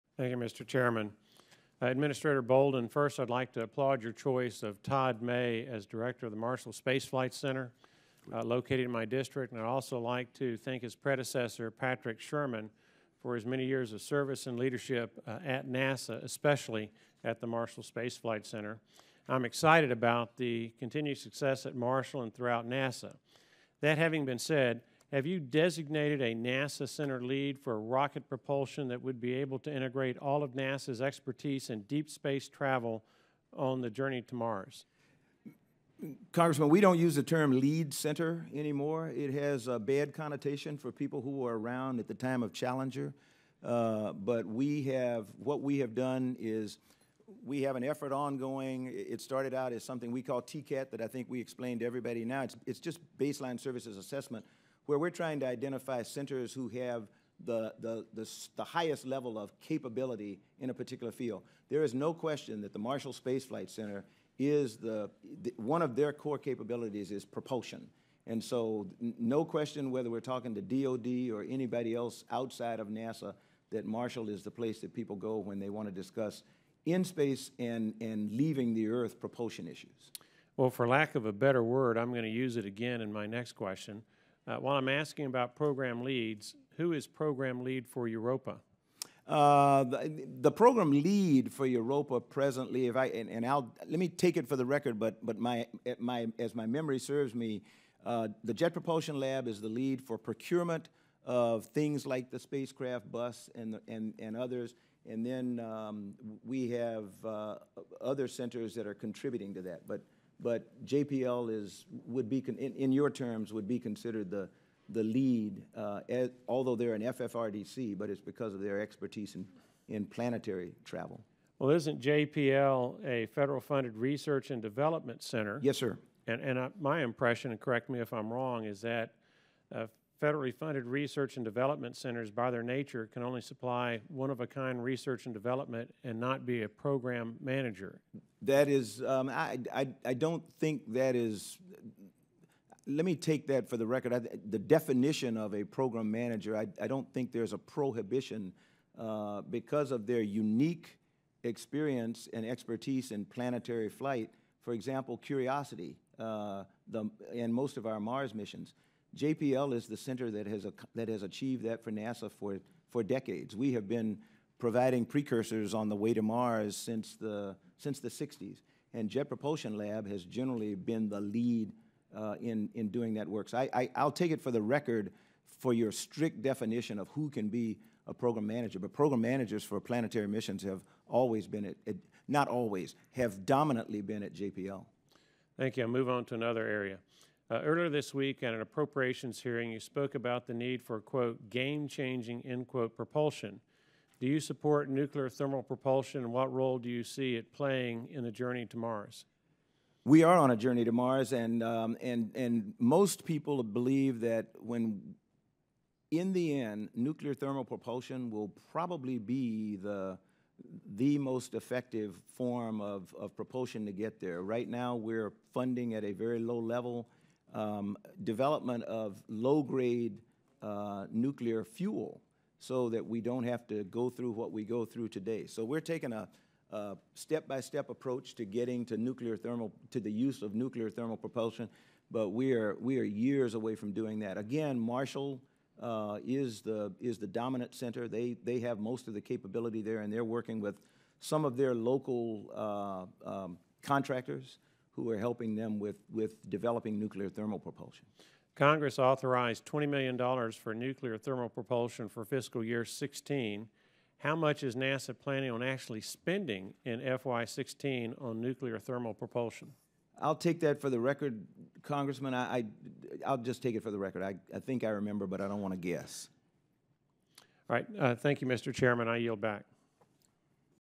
Rep. Brooks Questions Administrator Bolden on Budget Proposal for NASA for Fiscal Year 2017
Thursday, March 17, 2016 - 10:00am | 2318 Rayburn House Office Building